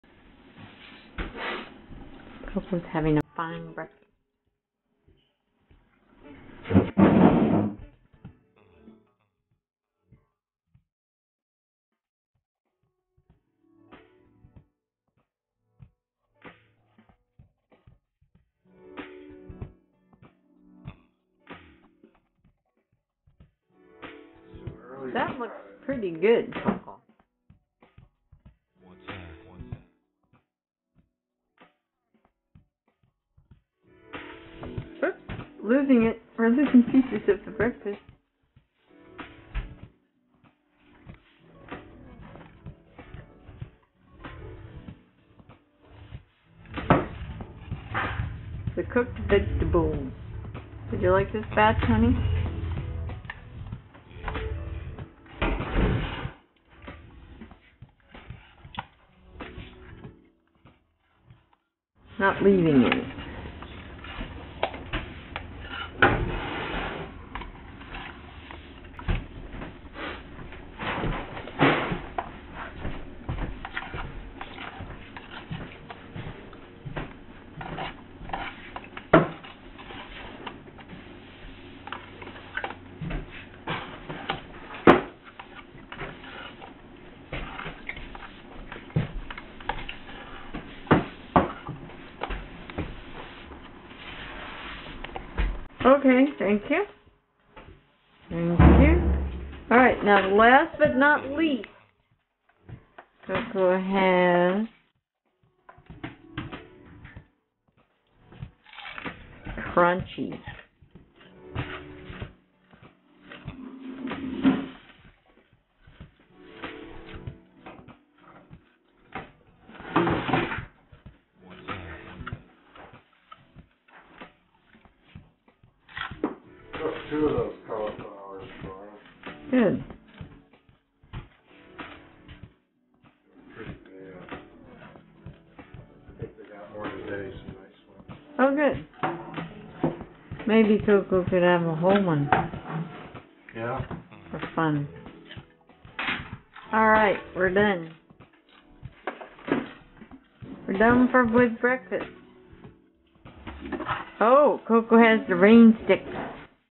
Koko Eats All Her Veggies
Koko eats a breakfast largely comprised of vegetables, and finishes every last bit. She starts with a plastic container full of cooked, mixed vegetables, which she delicately eats with a spoon. Then she proceeds to crunch on a hand full of string beans, before calling it a breakfast. Ron (Dr. Ron Cohn, co-founder, VP and photo/video-documentarian) can be heard softly in the background talking about some nice-looking donated cauliflowers that Koko may be sampling later that day (for lunch). Penny (Dr. Penny Patterson, co-founder, President and Director of Research) takes this video with a hand-held camera, while commenting on Koko\\\'s vigorous and single-minded approach to breakfast. Her comments are also included her as subtitles.